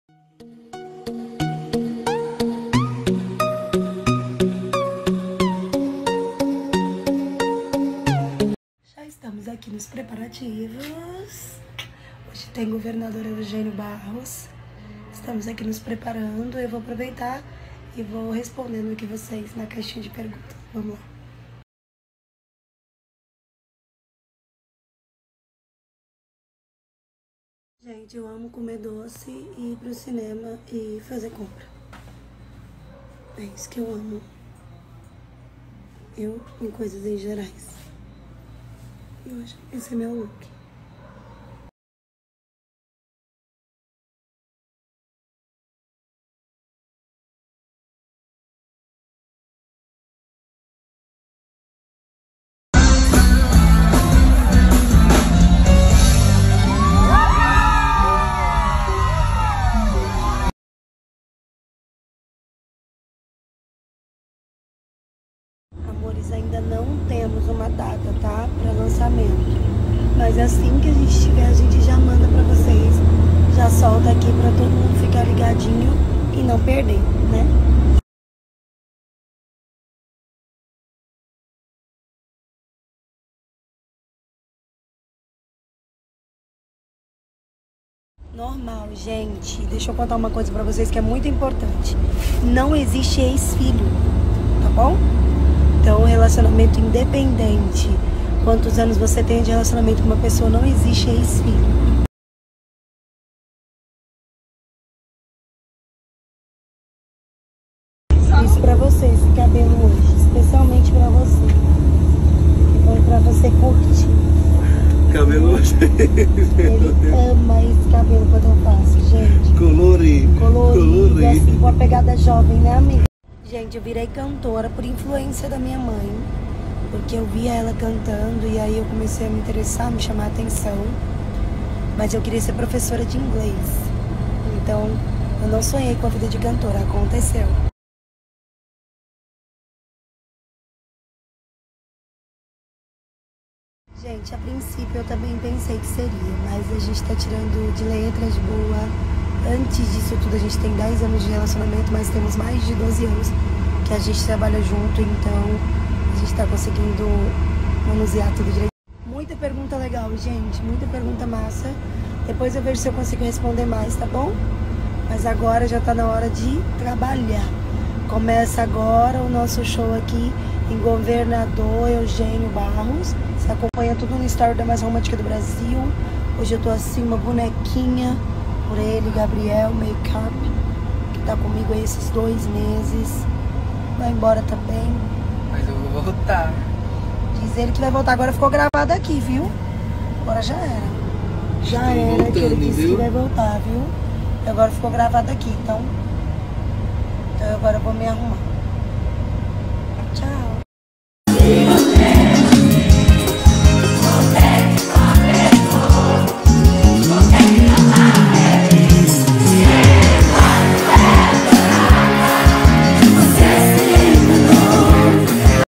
Yara Tchê respondendo perguntas dos fãs | Central da Desejo
Gostou desse vídeo e quer acompanhar por aqui tudo que acontece no Instagram da Desejo de Menina?